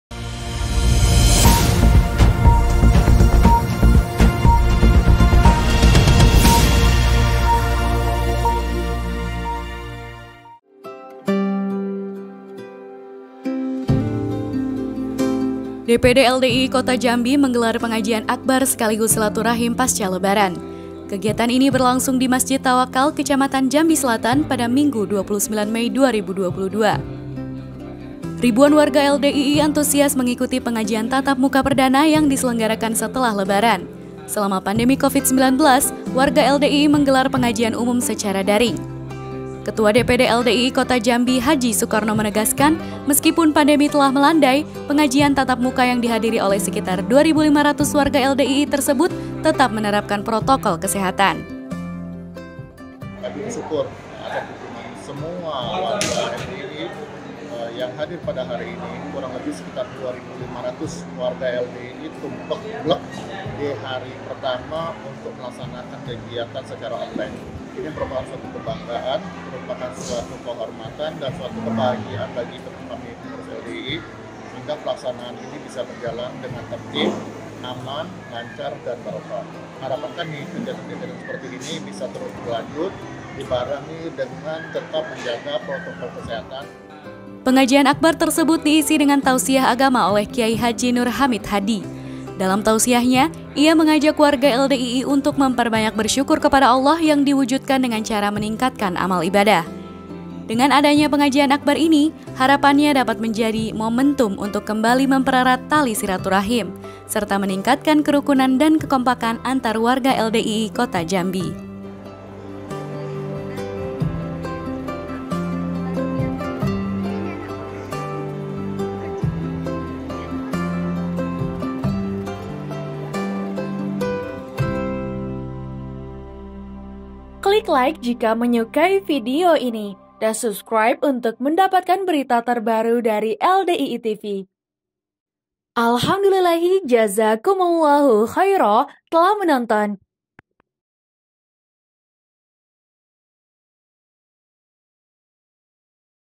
LDII TV: Ribuan Warga LDII Kota Jambi Ikuti Pengajian Akbar
DPD LDII Kota Jambi menggelar Pengajian Akbar Sekaligus Silaturahim Pasca Lebaran. kegiatan ini berlangsung di Masjid Tawakkal kecamatan jambi selatan pada minggu 29 Mei 2022.

Ribuan warga LDII antusias mengikuti pengajian tatap muka perdana yang diselenggarakan setelah lebaran. Selama pandemi covid-19, warga LDII menggelar pengajian umum secara daring.

Ketua DPD LDII Kota Jambi, H. Sukarno menegaskan, meskipun pandemi telah melandai, pengajian tatap muka yang dihadiri oleh sekitar 2500 warga LDII tersebut, tetap menerapkan protokol kesehatan.

Facebook : Lembaga Dakwah Islam Indonesia